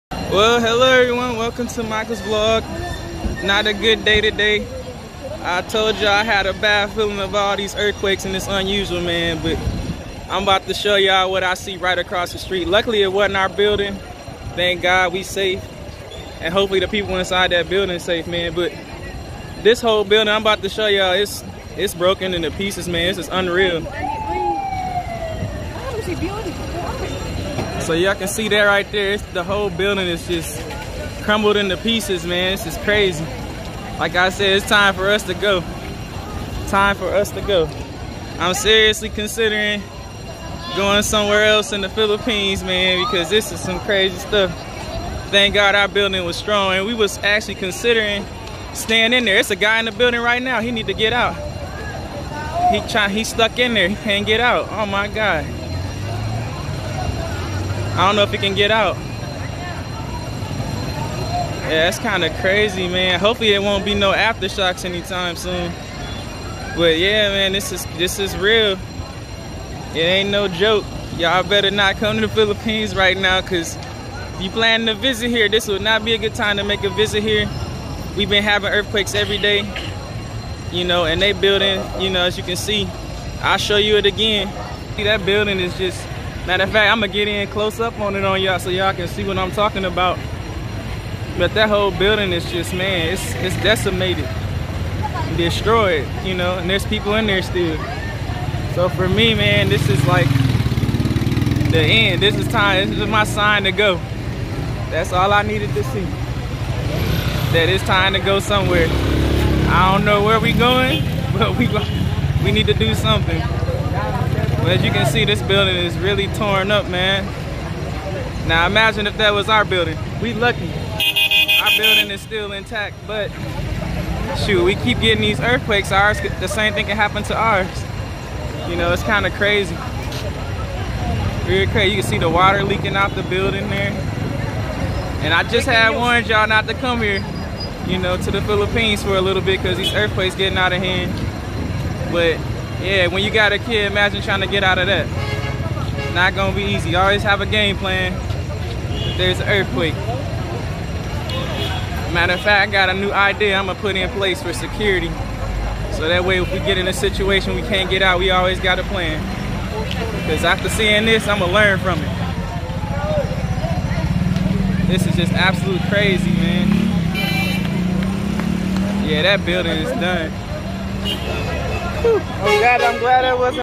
Earthquake October 31st 2019 Damage
Another earthquake occurred in Davao Philippines and severly damaged building that was behind our place of stay.